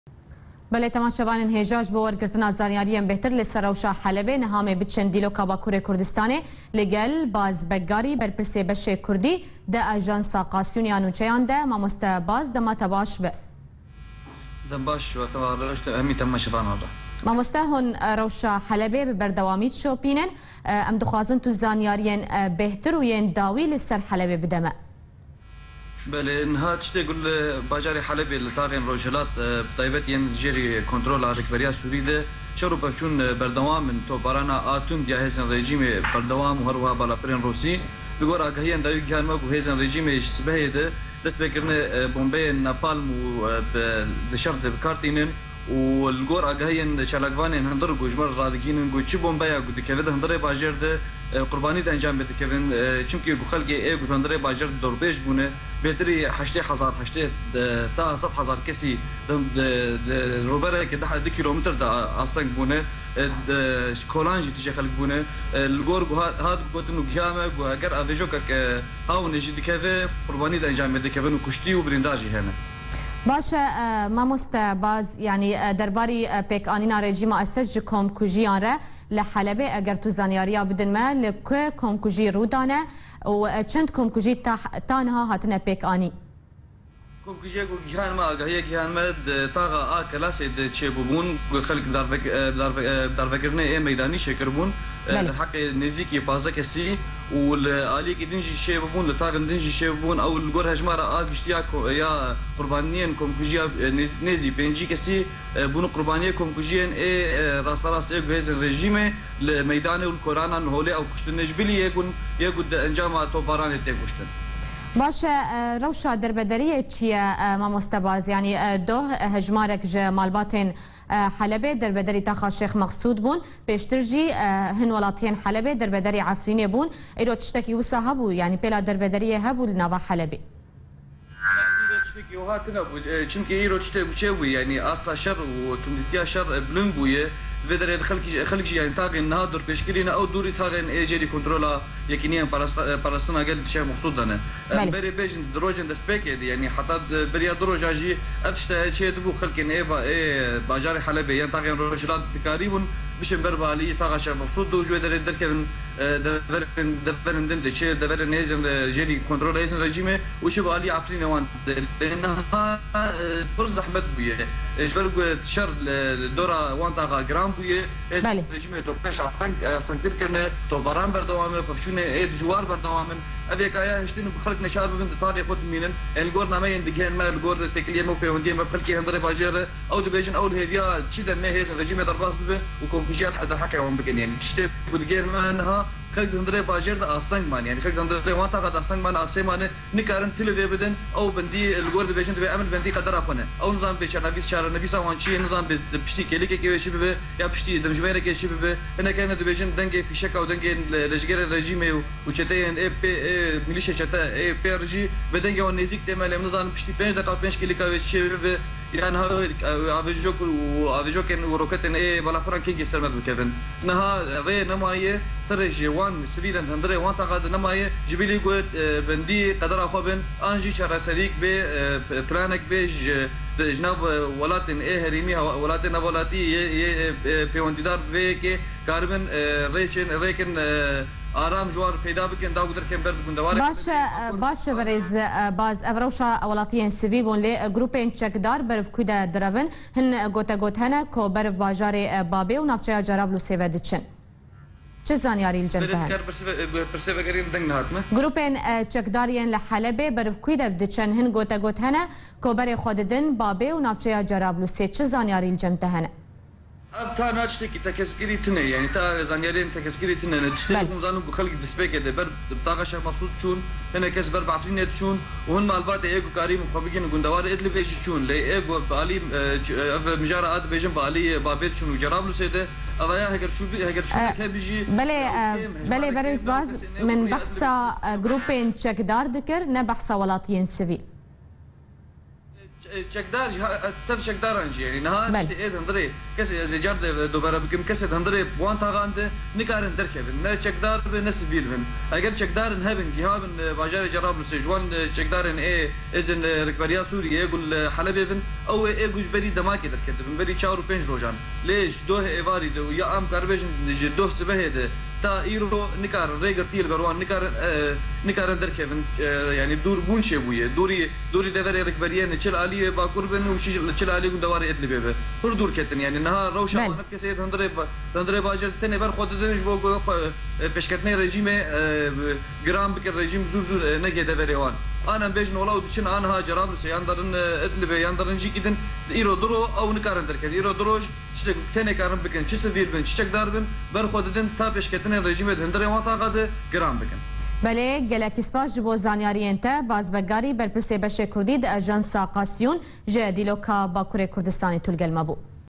BAZ BEGARÎ... Rojnamevan
HELEB …BANGÊN HEWARÇÛNÊN DI NAVA TAXÊN ROJHILATÊ HELEBÊ DE DENG DIDE